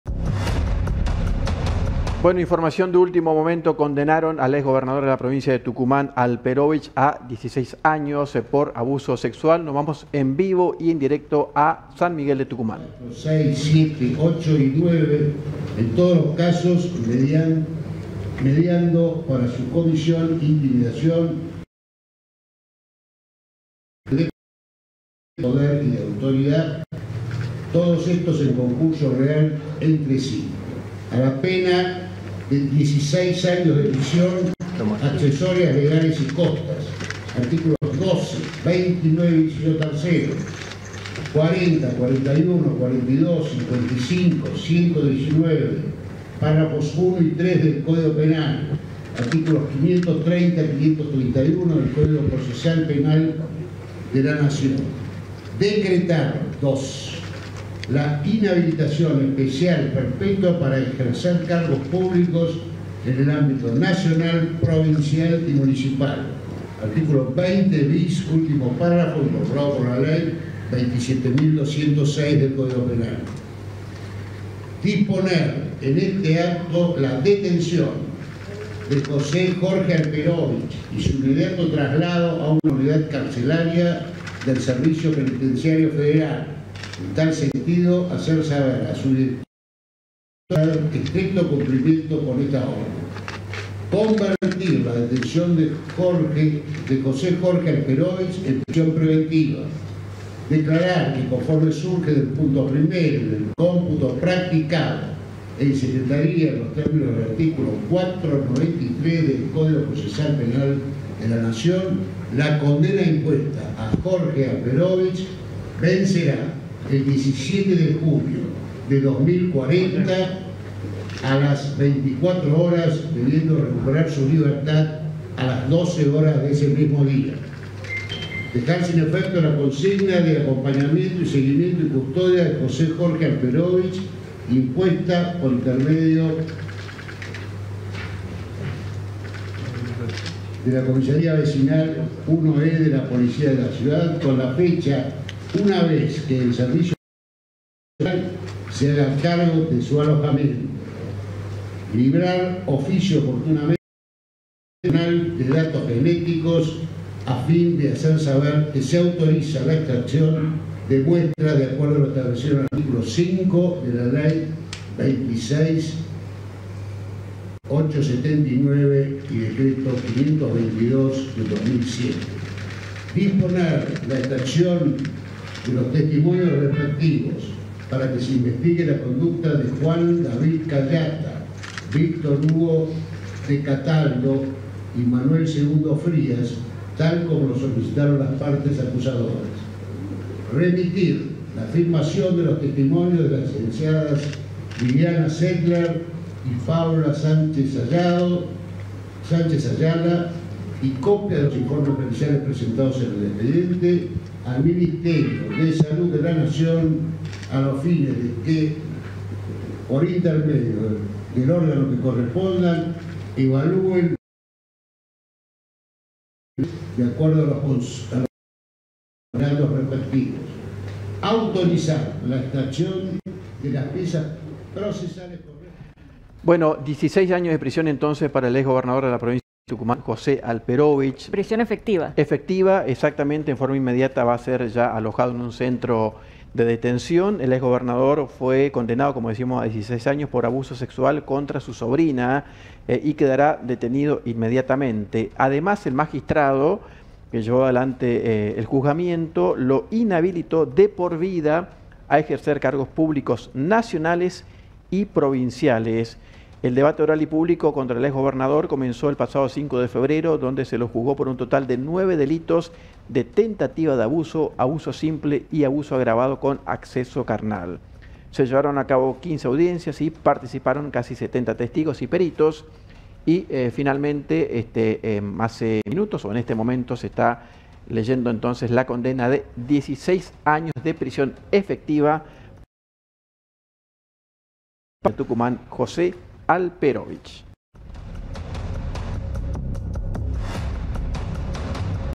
NGFEDERAL - CONDENAN POR ABUSO SEXUAL AL EX GOBERNADOR DE TUCUMÁN
CONDENAN POR ABUSO SEXUAL AL EX GOBERNADOR DE TUCUMÁN

16 AÑOS DE PRISIÓN PARA JOSÉ ALPEROVICH